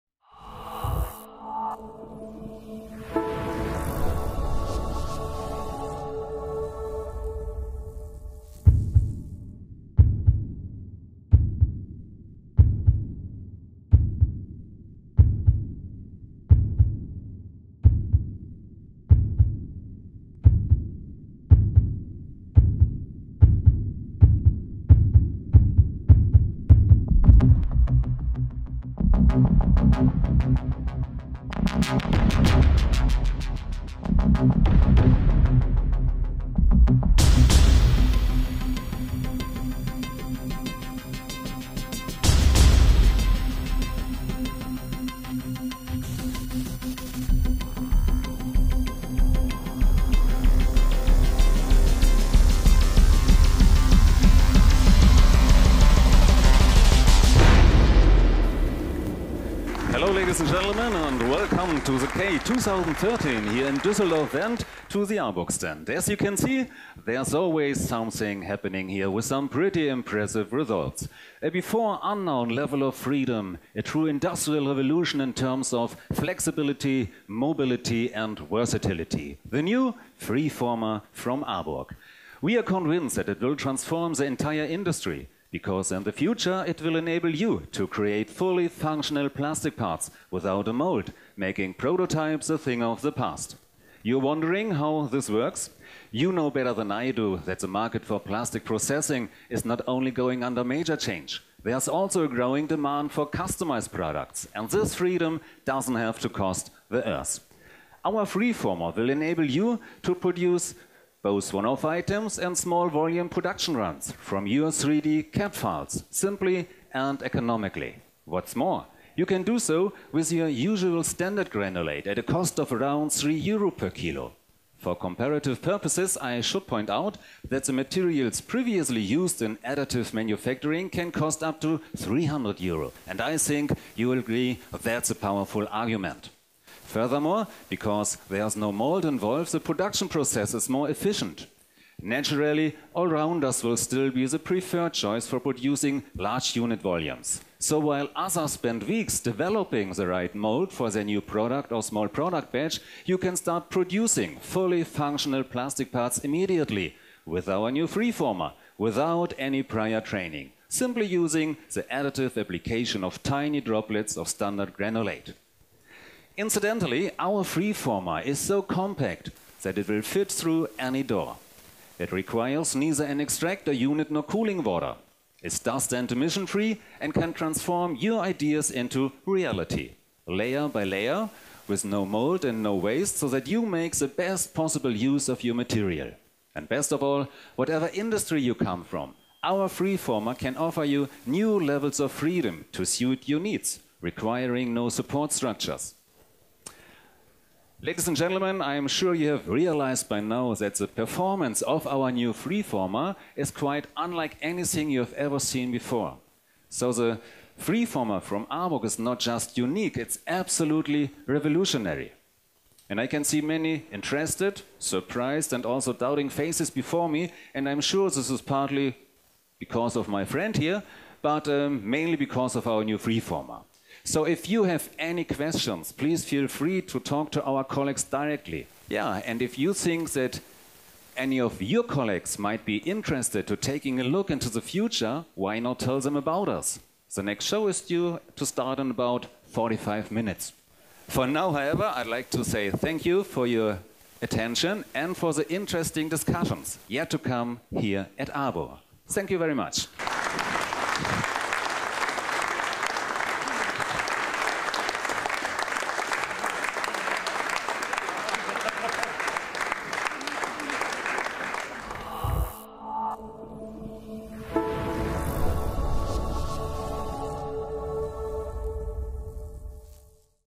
K 2013: showtime for the freeformer
- At the K 2013 ARBURG is are presenting the world premiere of the freeformer and ARBURG Material Freeforming (AMF). The show marking the occasion is just as excellent as the revolutionary product idea and the process for additive manufacturing! From 16 to 23 October, the "flexible" artistes are performing every 45 minutes.

The fact that ARBURG is always making things happen is demonstrated by the incomparable freeformer show at the K 2013 in Hall 13 at Stand A13. With the most baffling contortions, our artistes illustrate the freedom and flexibility provided by our unique additive manufacturing system.

K 2013: Showtime für den freeformer

- Auf der K 2013 präsentiert ARBURG weltweit erstmals den freeformer und das ARBURG Material Freiformen (AMF). So gut wie die revolutionäre Produktidee und das Verfahren zur additiven Fertigung ist auch die Show dazu! Die „flexiblen" Künstlerinnen sind vom 16. bis 23. Oktober alle 45 Minuten zu sehen.

Dass sich bei ARBURG immer etwas bewegt, zeigte die unvergleichliche freeformer Show auf der K 2013 in Halle 13 an Stand A13. So genannte Kontorsionskünstlerinnen veranschaulichen durch schier unmögliche Bewegungen die Freiheit und Flexibilität, die unser einzigartiges System zur additiven Fertigung bietet.